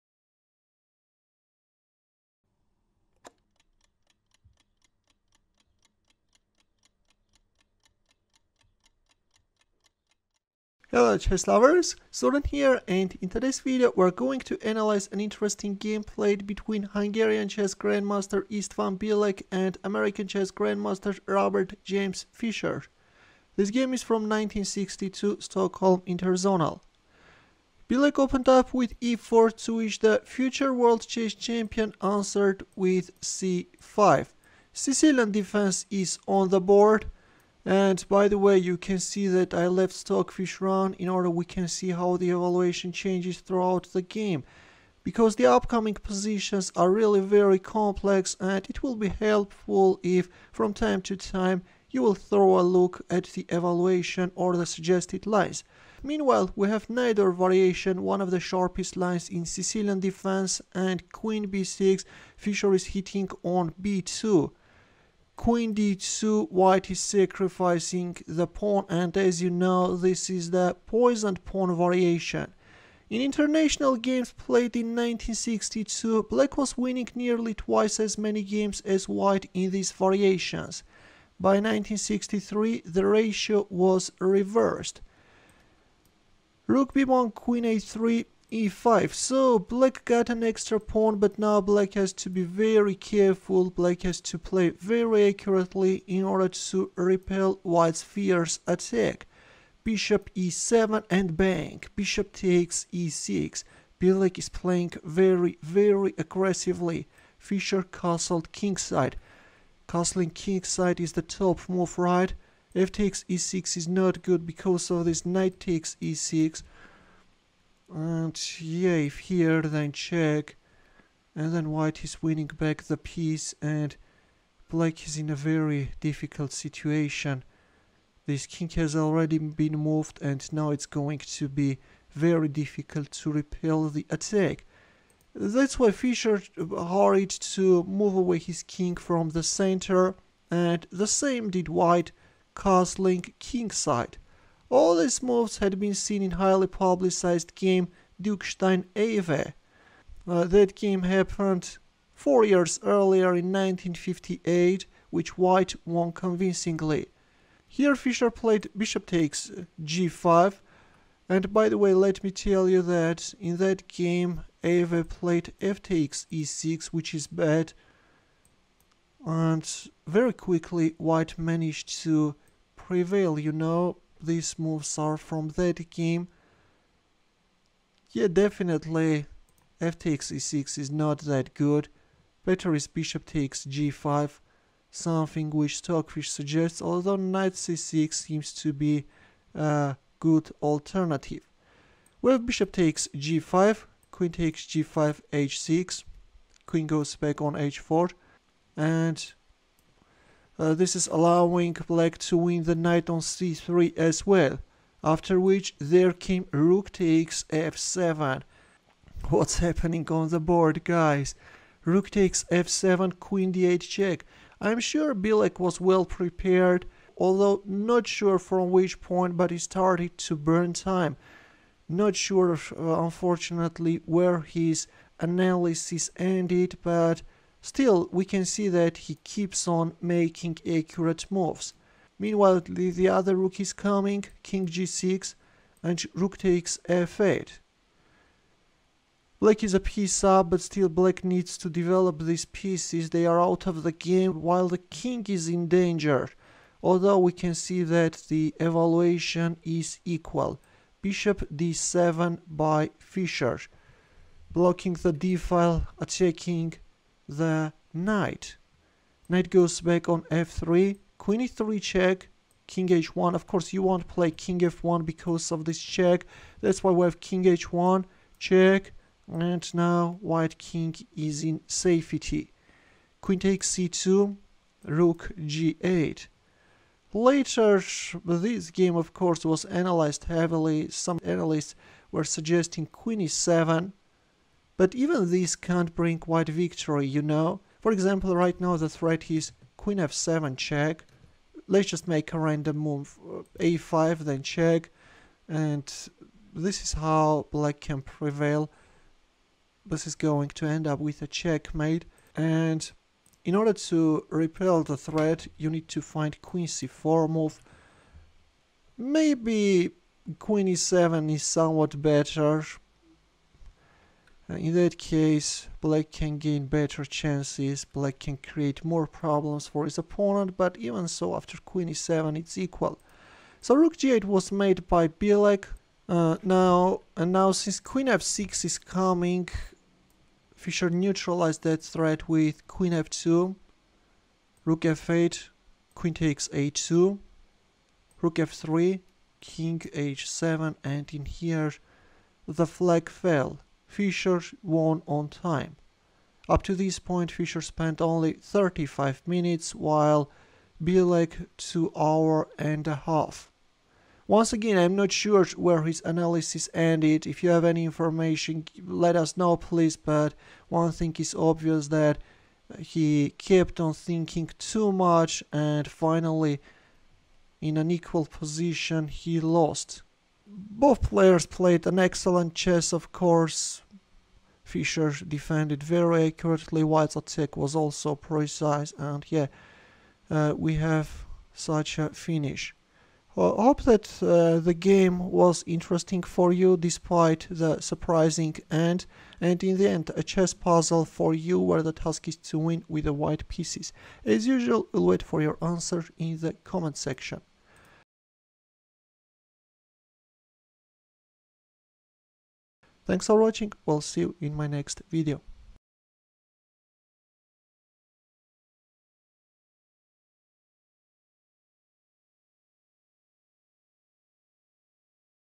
Fischer Passes Alive Through The Hell Of Poisoned Pawn Variation
Bobby Fischer plays the Poisoned Variation and manages to survive while facing Istvan Bilek's stormy attack!
Istvan Bilek vs Robert James Fischer
Stockholm Interzonal (1962), Stockholm SWE, rd 5, Feb-03
Sicilian Defense: Najdorf Variation. Poisoned Pawn Accepted (B97)
1.e4 c5 2.Nf3 d6 3.d4 cxd4 4.Nxd4 Nf6 5.Nc3 a6 6.Bg5 e6 7.f4
Qb6 8.Qd2 Qxb2 9.Rb1 Qa3 10.e5 dxe5 11.fxe5 Nfd7 12.Bc4 Be7
13.Bxe6 O-O 14.O-O Bxg5 15.Qxg5 h6 16.Qh4 Qxc3 17.Rxf7 Rxf7
18.Qd8+ Nf8 19.Bxf7+ Kxf7 20.Rf1+ Kg6 21.Rxf8 Bd7 22.Nf3 Qe3+
23.Kh1 Qc1+ 24.Ng1 Qxc2 25.Rg8 Qf2 26.Rf8 Qxa2 27.Rf3 Kh7
 0-1